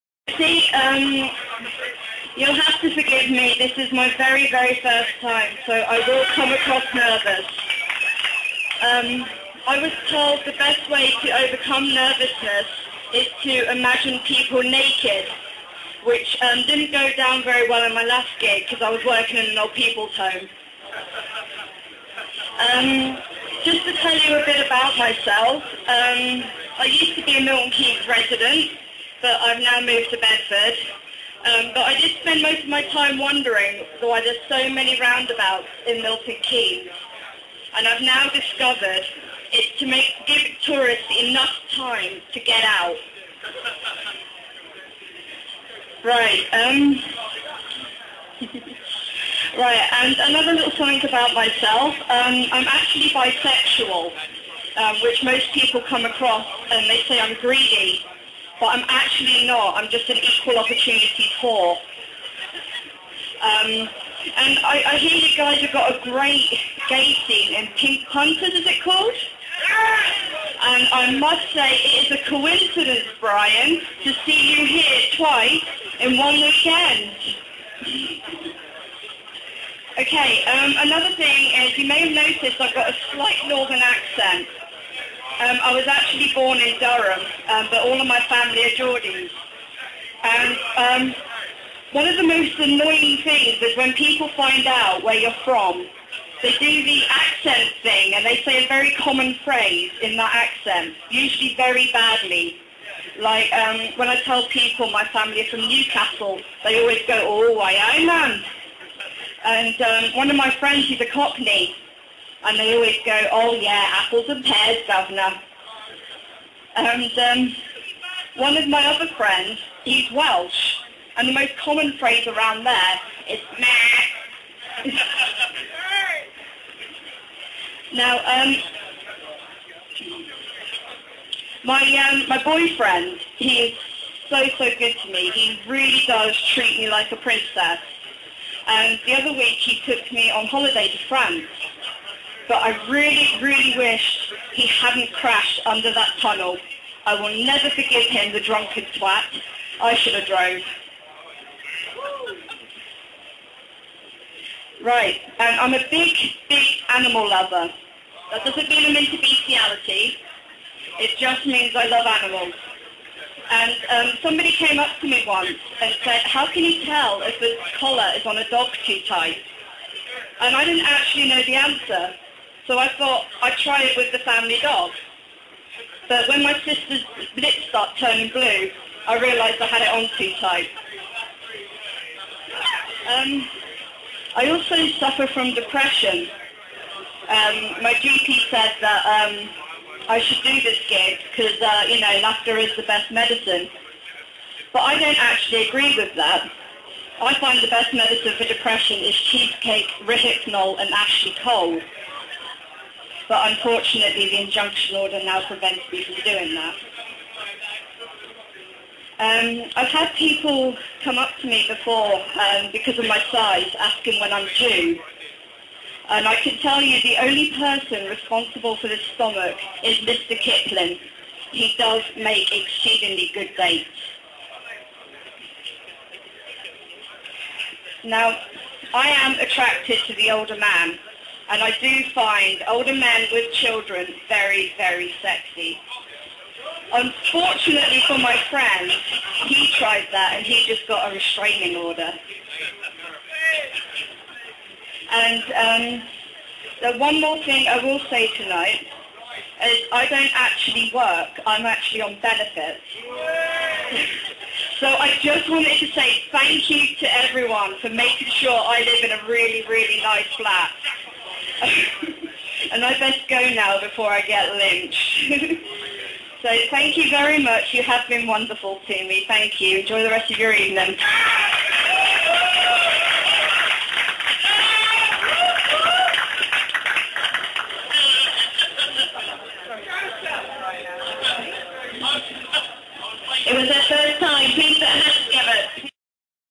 Kryssi Keightley Live At The Sno:Bar Xscape Milton Keynes July 4th 2010
Kryssi Keightley's very first gig, recorded on my mobile live at the Sno:Bar in Milton Keynes.
This is the 2nd time i've uploaded this because the original is filmed at a 90 degree angle. I've rotated this and altered the sound levels slightly. Hope you enjoy it, Barnaby.